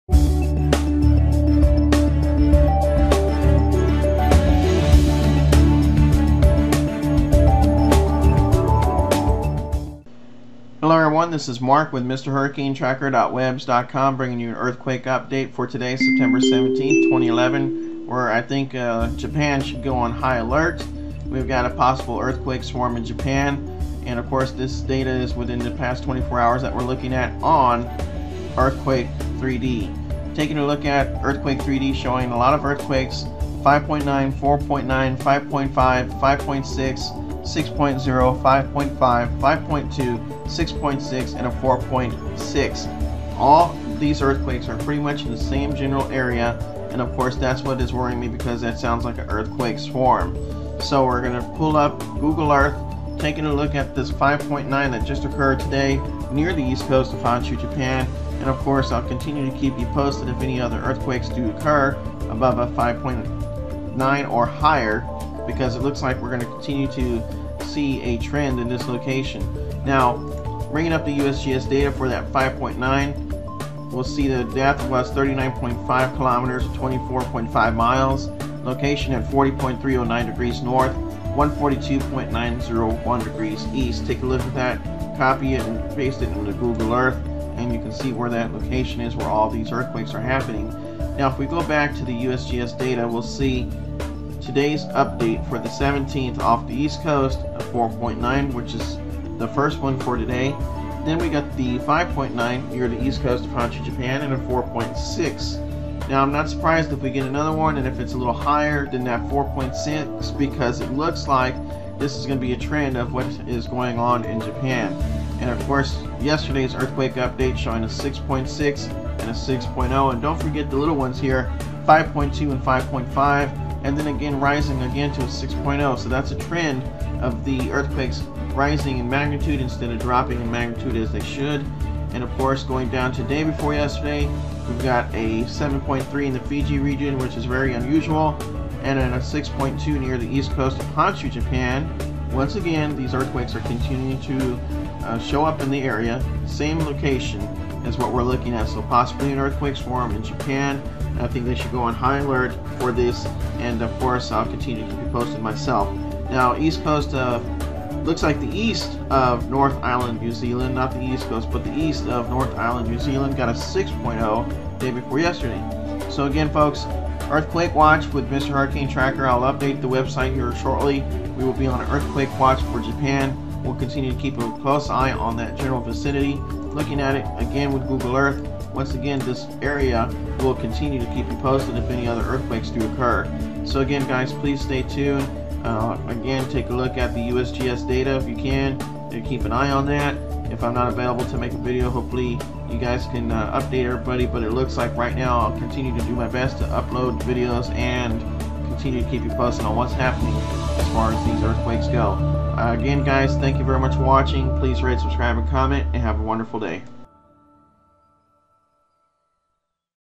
Earthquake Swarm in Japan Earthquake Alert
Earthquakes continue in Japan with 5.0 and greater.  These Earthquakes are in a general area and is causing an Earthquake Swarm.  Look up the USGS for more data.  The 5.9 that struck today could indicate that a stronger quake is on its way.

Magnitude 5.9
Date-Time Saturday, September 17, 2011 at 07:33:59 UTC
Saturday, September 17, 2011 at 04:33:59 PM at epicenter
Time of Earthquake in other Time Zones

Depth 39.5 km (24.5 miles)
Region NEAR THE EAST COAST OF HONSHU, JAPAN
Distances 122 km (75 miles) E of Hachinohe, Honshu, Japan
164 km (101 miles) ENE of Morioka, Honshu, Japan
190 km (118 miles) ESE of Aomori, Honshu, Japan
583 km (362 miles) NNE of TOKYO, Japan

Location Uncertainty horizontal +/- 17.4 km (10.8 miles); depth +/- 8.5 km (5.3 miles)
Parameters NST=368, Nph=380, Dmin=191.5 km, Rmss=0.62 sec, Gp=112°,
M-type=regional moment magnitude (Mw), Version=7
Source Magnitude: USGS NEIC (WDCS-D)
Location: USGS NEIC (WDCS-D)

Event ID usc0005w44